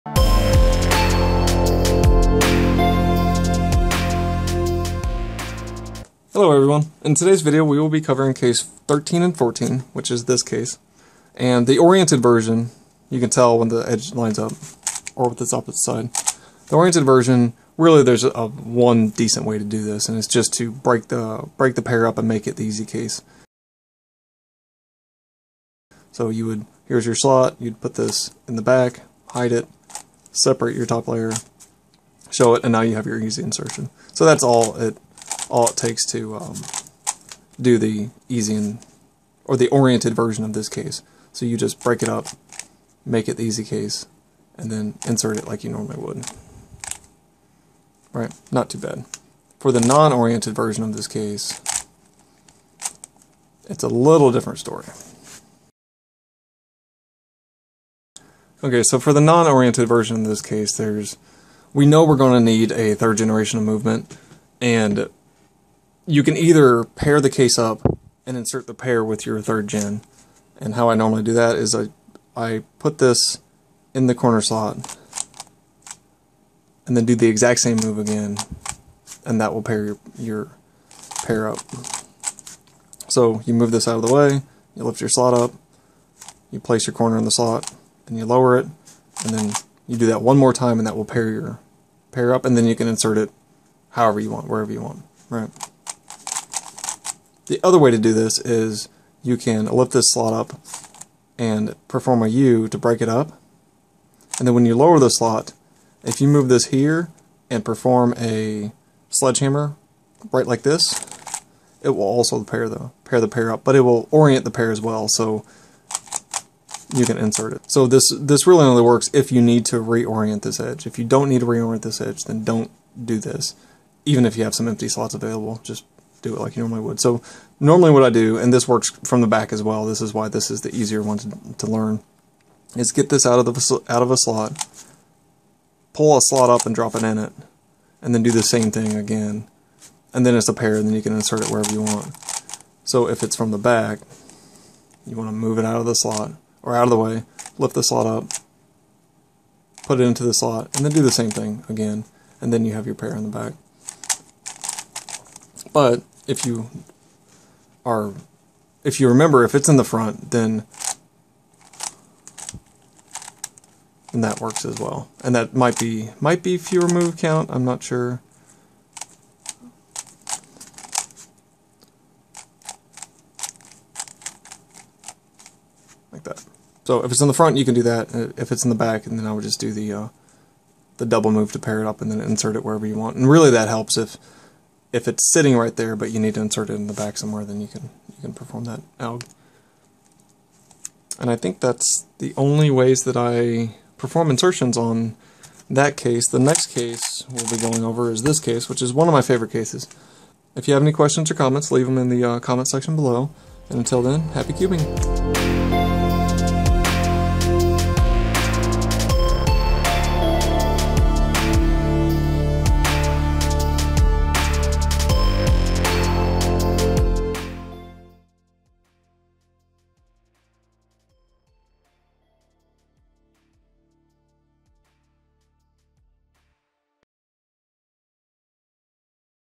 Advanced F2L Case #13 - 14
Advanced F2L: Overview of Case #13 and Case #14 from the speed solving wiki

Let me know if you have any questions or comments below.

Happy Cubing! - Caleb Miller.